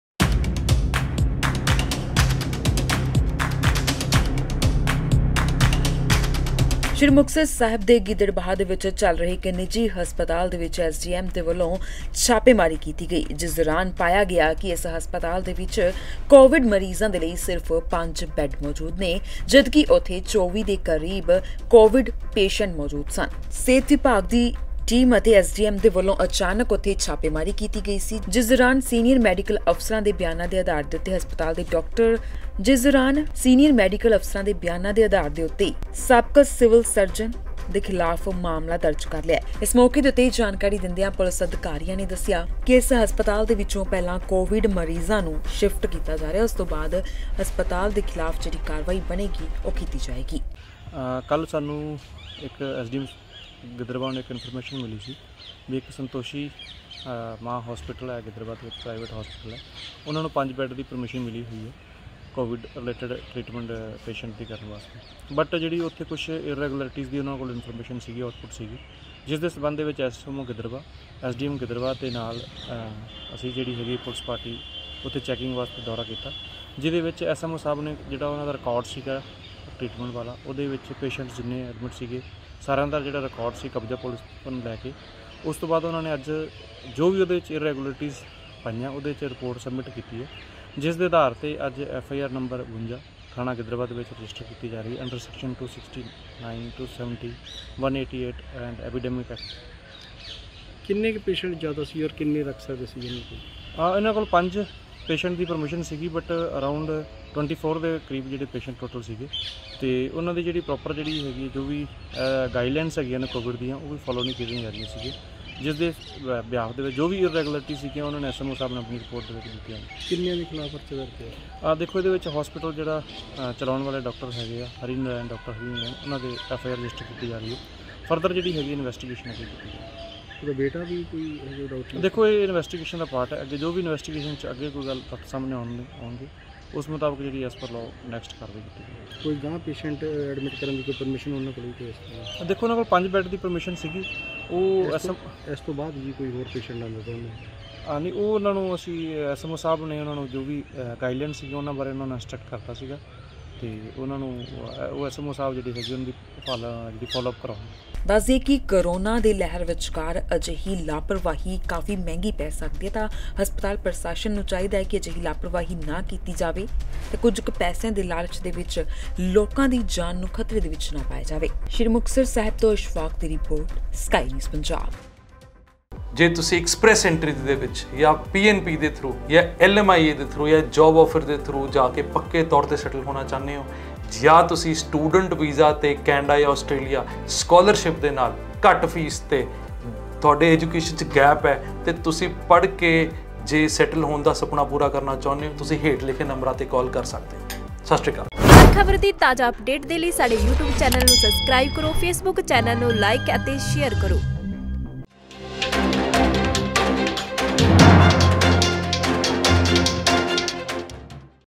ਆਹ ਵੇਖਲੋ ਨਿੱਜੀ Hospital ਦੇ ਹਾਲ, 5 Covid Beds ਵਾਲੇ ਹਸਪਤਾਲ ‘ਚ ਭਰੇ ਨੇ 24 ਮਰੀਜ਼!
ਆਹ ਵੇਖਲੋ ਨਿੱਜੀ Hospital ਦੇ ਹਾਲ,
5 Covid Beds ਵਾਲੇ ਹਸਪਤਾਲ ‘ਚ ਭਰੇ ਨੇ 24 ਮਰੀਜ਼!